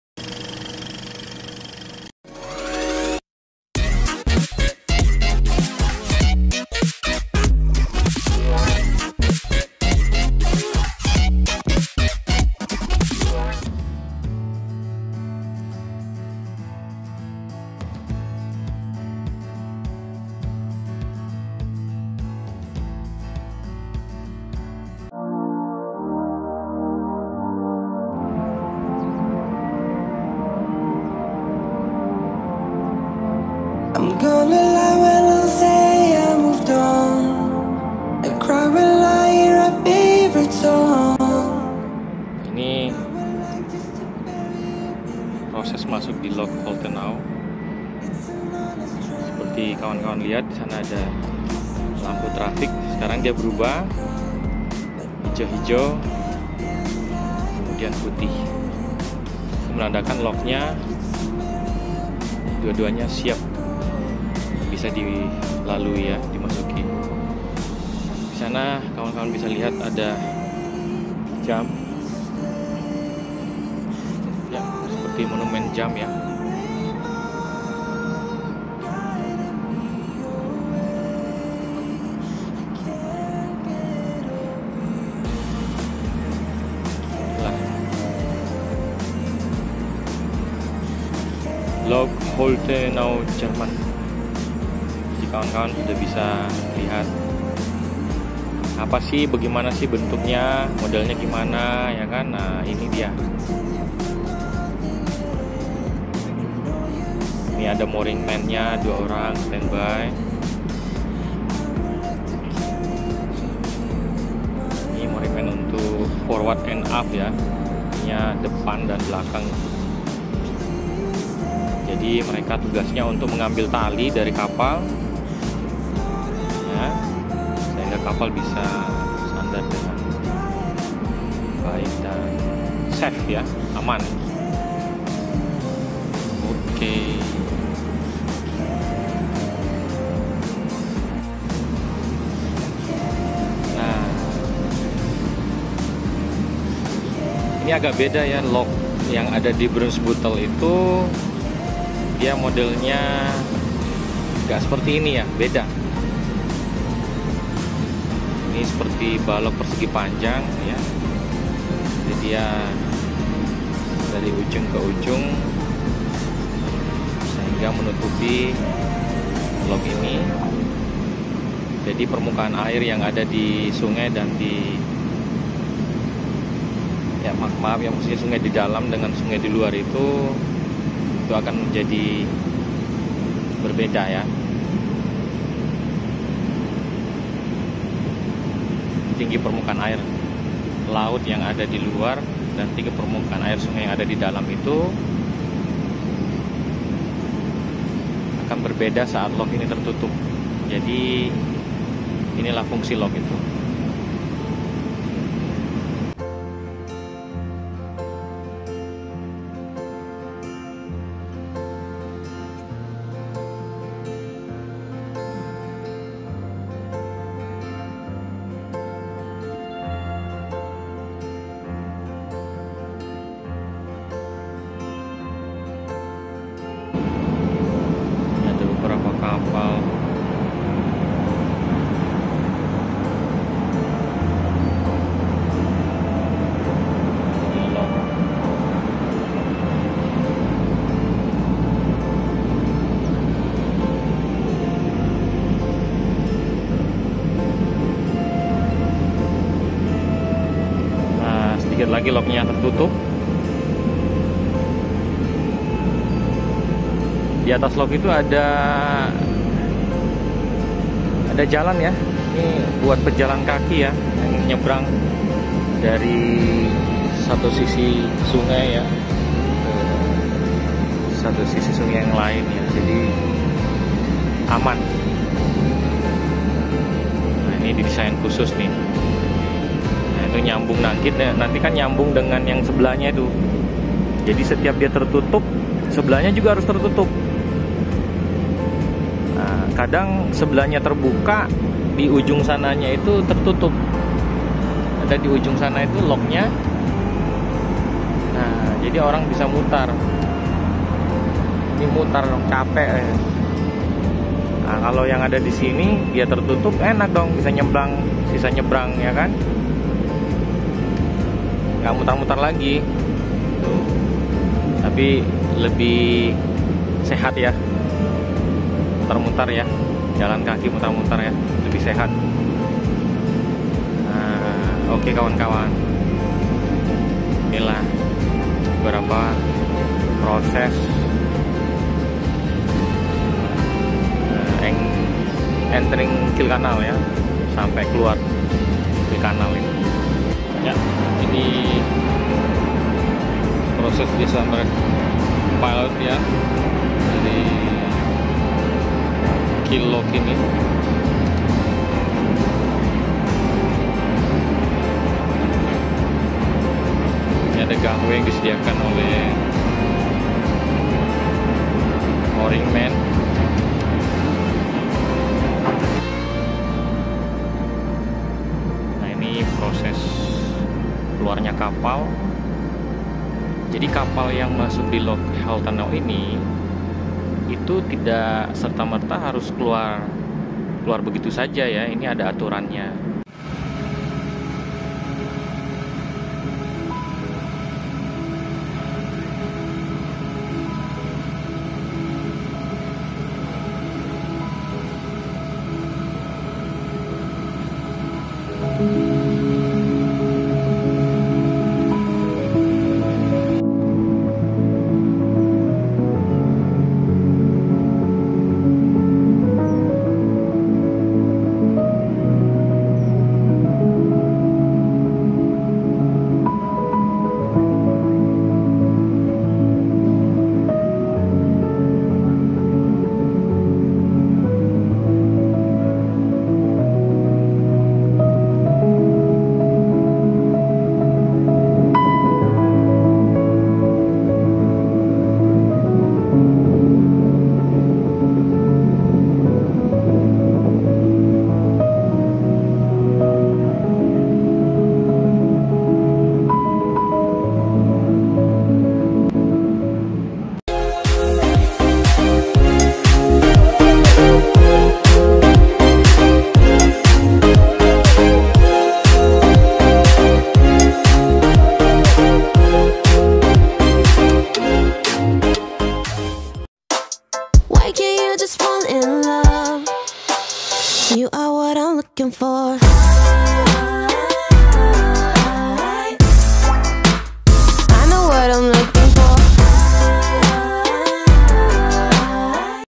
Melihat Proses Penutupan River Lock Kiel Canal Di Negara Jerman
Divideo Ini saya Buatkan agar sahabatku bisa mengetahui View dari Negara Jerman pada Kiel Canal.
Semoga Bermanfaat dan Menjadi Motivasi Menjadi Seorang Pelaut Profesional.